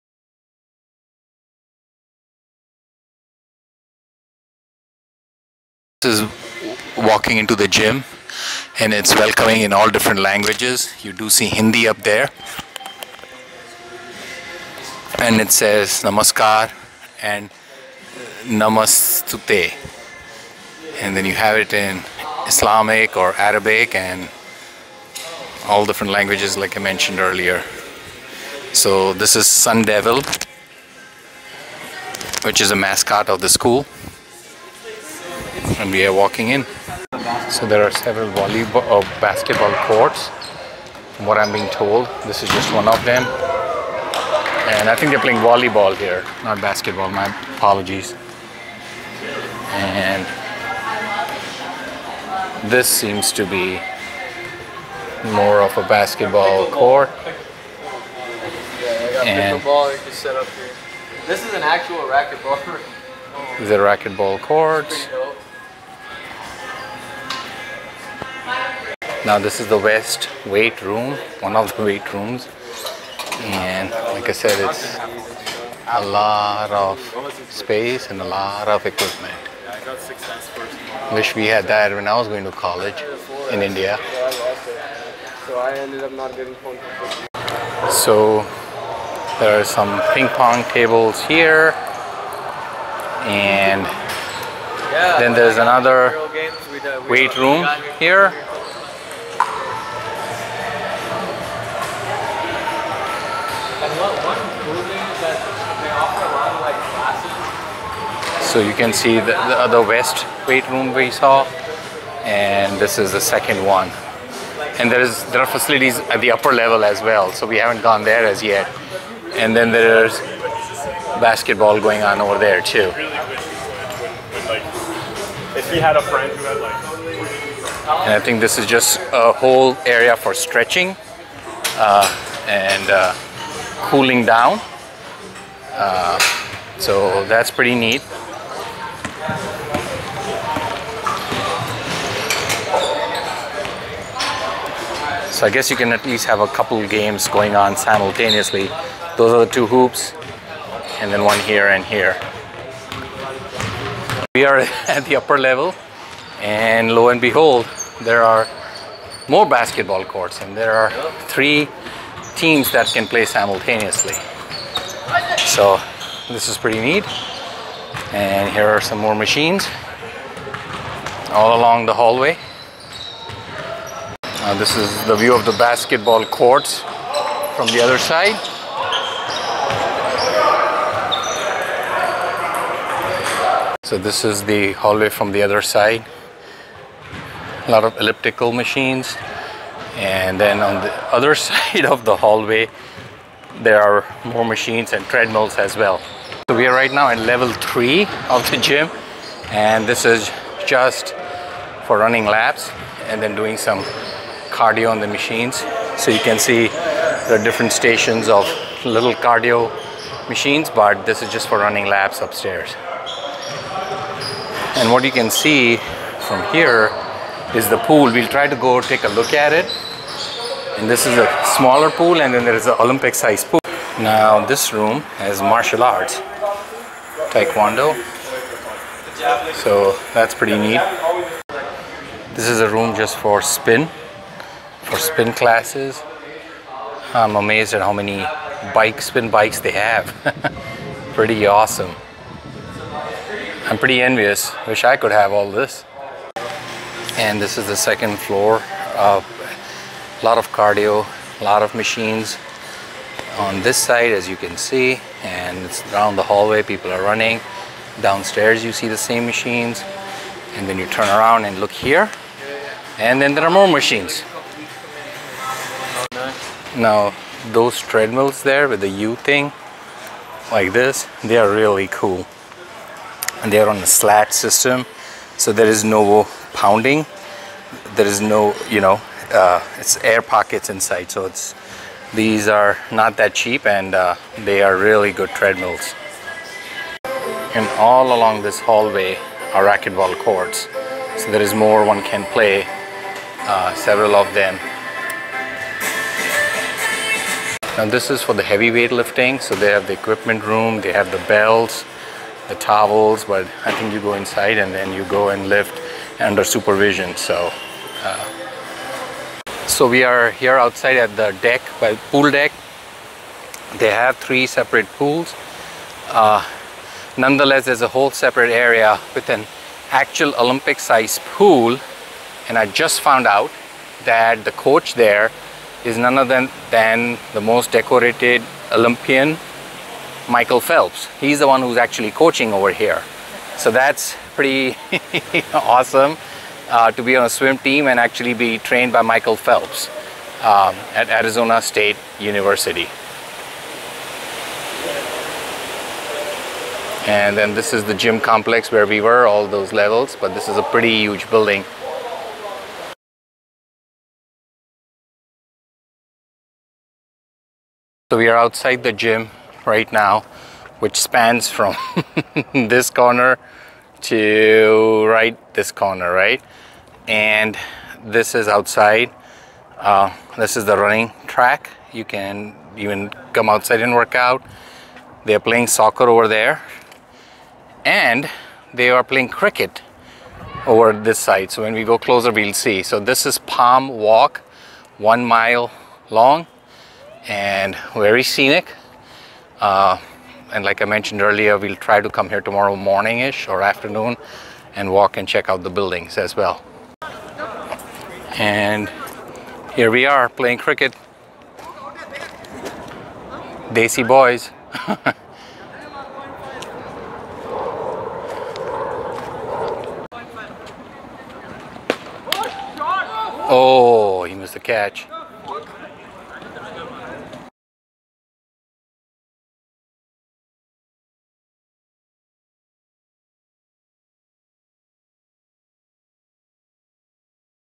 Walk Through Arizona State University (ASU's) Tempe Campus - Part 2 : Recreational Center
A quick tour of ASU's 3 storied recreational center that spans over 6 acres.

Eating a healthy diet and staying active have a significant impact on your physical health and overall well-being.

At ASU’s fitness & wellness center, students can take advantage of individual and group fitness classes to outdoor adventures and aquatics; and from intramural and club sports to health and well-being classes, there’s something for everyone no matter level of ability or expertise.